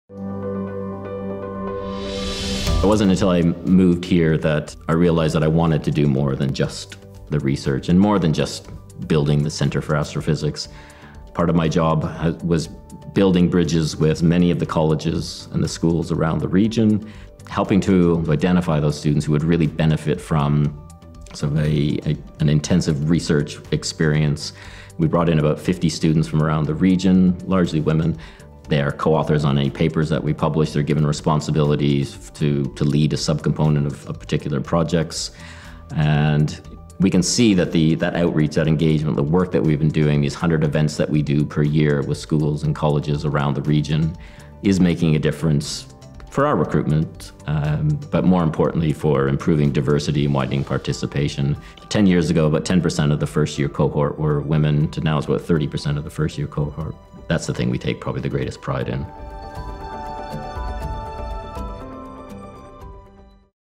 Research Impact at Hull - Promoting Justice and Fairness
Professor Brad Gibson describes how he is using research in Astrophysics to increase diversity and widen participation.

Research Area: Promoting Justice and Fairness for All